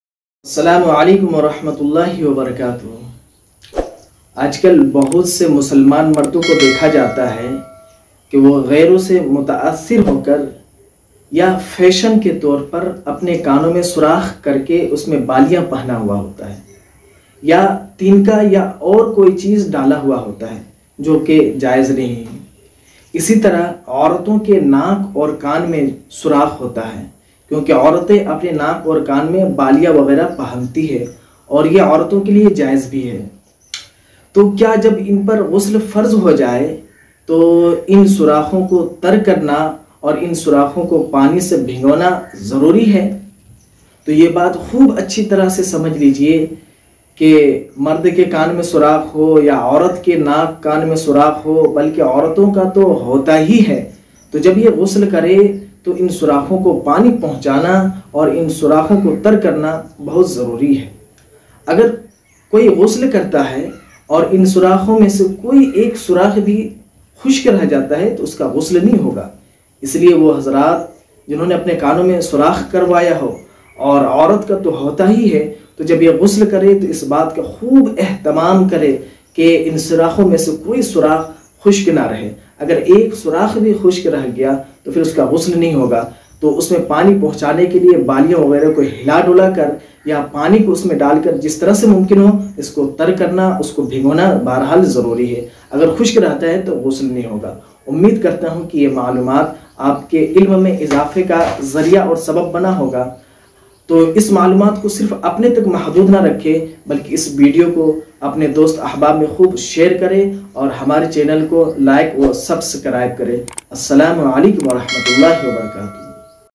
Kya gusal me nak kaan ke surakh me pani pahuchana jaruri hai ?? , Bahut aham jankari, Mufti Salman
Is video me gusal ka ek bahut hi aham masla bayan kiya gaya hai , nak kaan ka jo surakh hota hai jis me Bali wagera aurte pahanti hai isi tarah bahut se mard bhi apne kaan ka surakh kar ke baliya wagera pahanta hai,  to in surakhon ko gusal me tar krna jaruri hai, agar khushk rah gaya to gusal nahi hoga, isliye gusal karte howe is ka khas khayal rakhen...